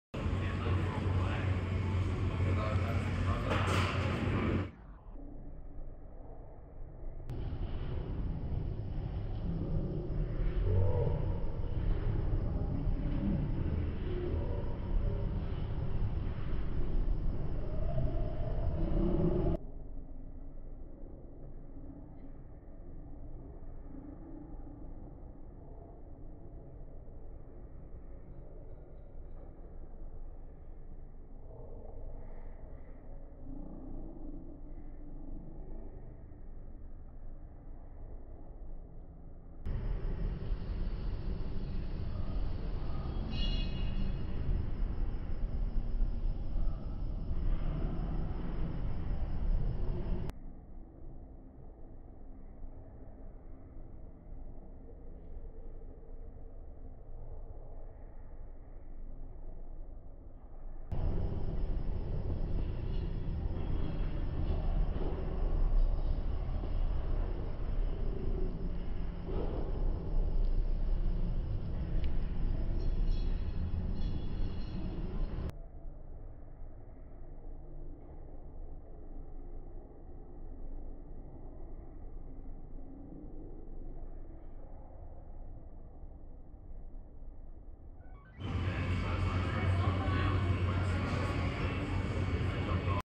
iPhone 16 Pro vs Samsung Galaxy S24 Ultra - 4K 120FPS Slow Motion Comparison
In this video, we will look at the 4K 120FPS (frames per second) slow motion video from both the iPhone 16 Pro (footage from the iPhone 16 Pro Max will be shown), as well as the Samsung Galaxy S24 Ultra. Let me know which one you prefer in the comments below. I hope you find this helpful!